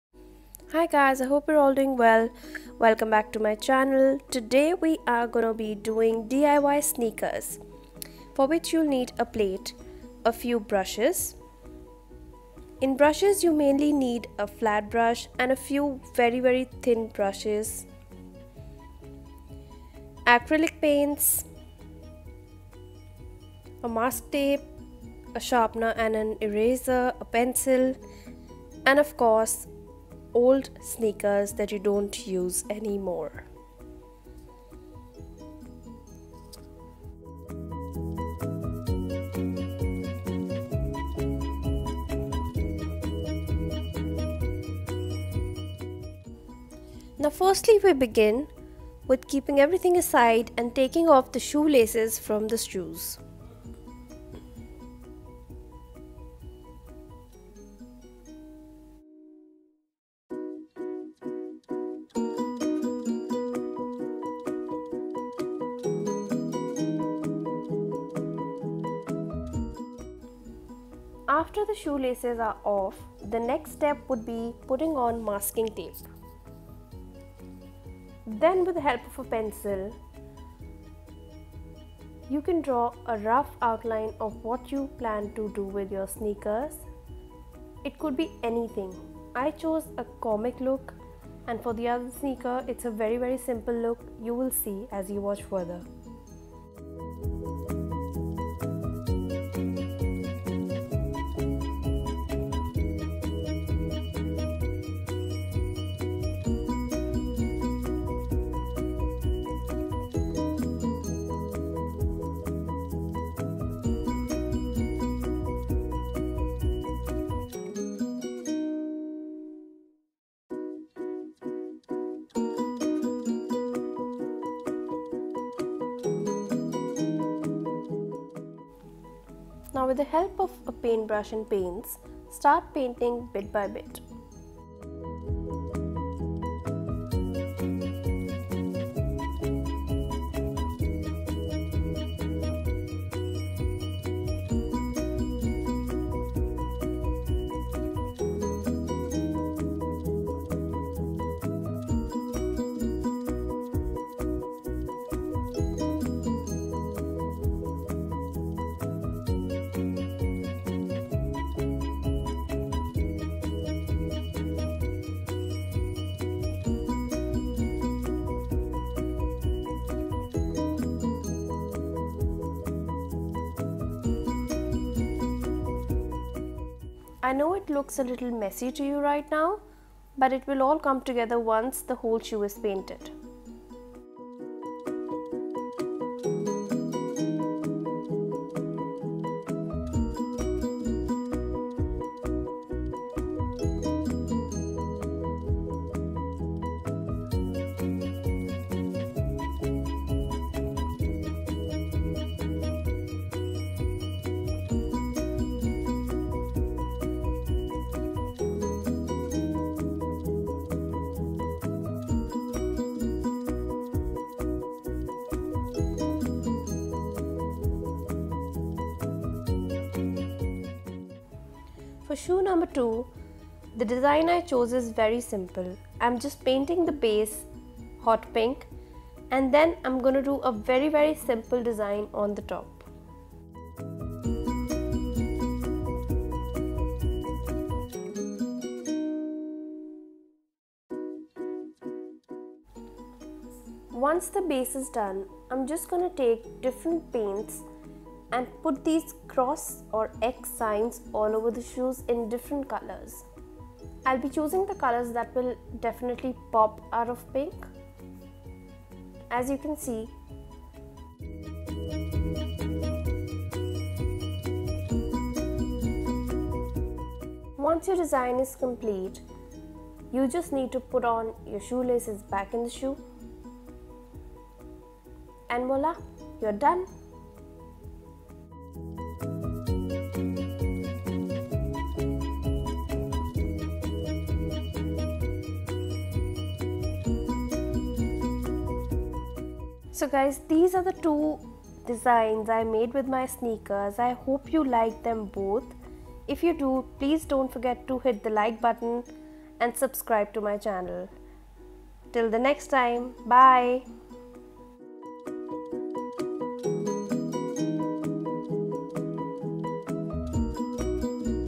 DIY POP ART SNEAKERS | 2 SUPER EASY STYLES | Mommy and Baby Bug
Hi guys
I hope you're doing well. Today I am doing a simple rough diy that any of you can do. I'm revamping my sneakers. I'm going to style  2 sneakers, one of each pair. Hope you like it. Im not an expert but did this just for fun.
Xoxo

TARANPREET
XOXO